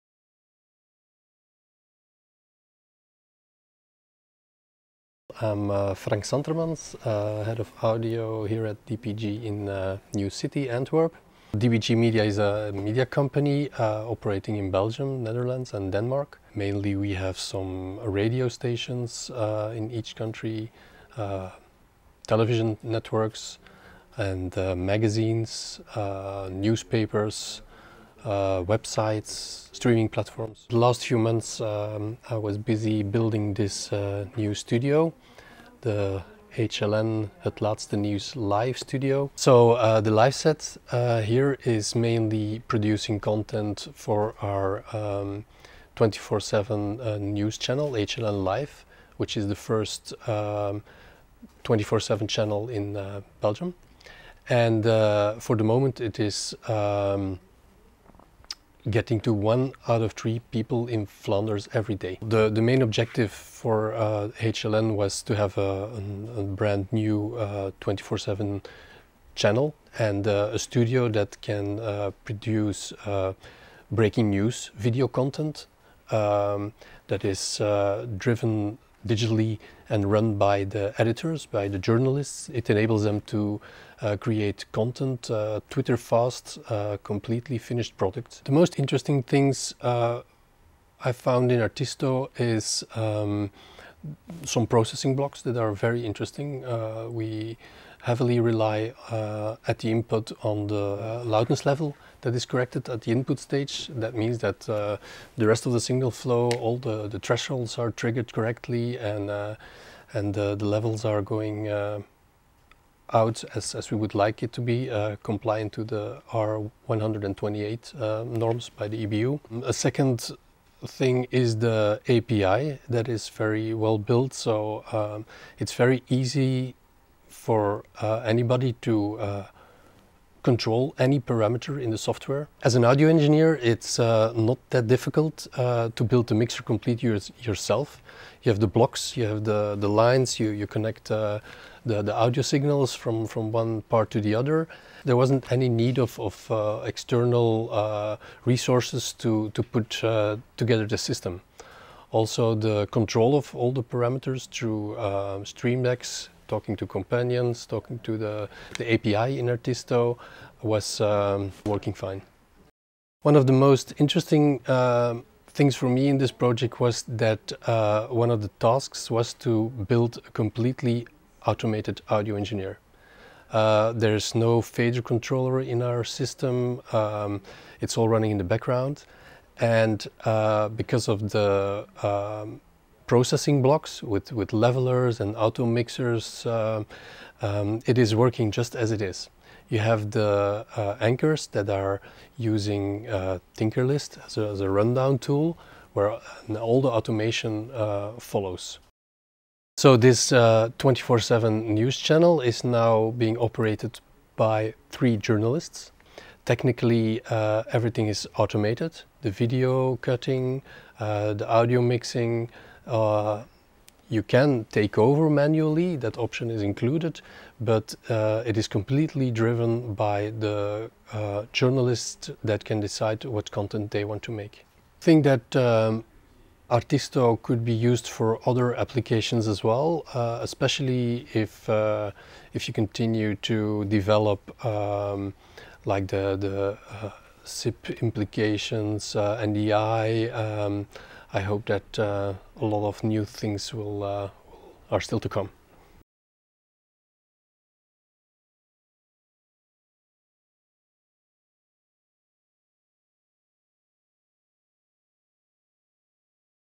Use Case: The Live Set by DPG Media
How and why DPG Media built an automated 24/7 news studio powered by 3 journalists thanks to artisto (also featuring @TinkerList Info )